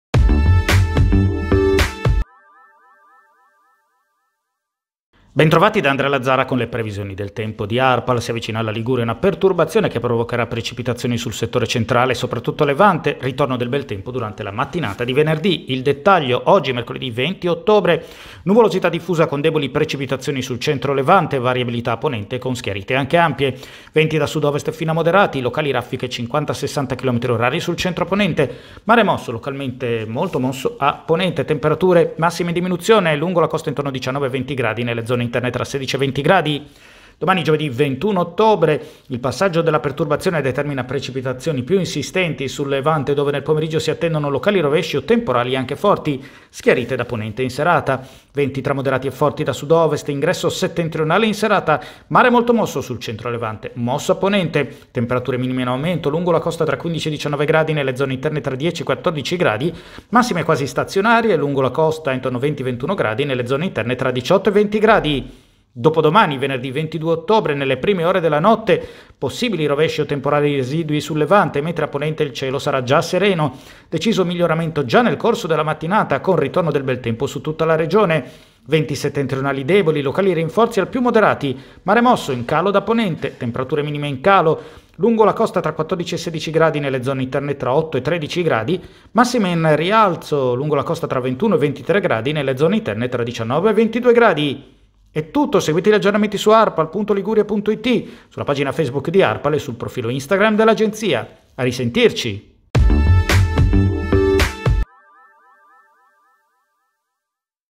20 ottobre 2021, le video previsioni del Centro Meteo Arpal
Si avvicina alla Liguria una perturbazione che provocherà precipitazioni sul settore centrale e soprattutto a Levante. Ritorno del bel tempo durante la mattinata di venerdì.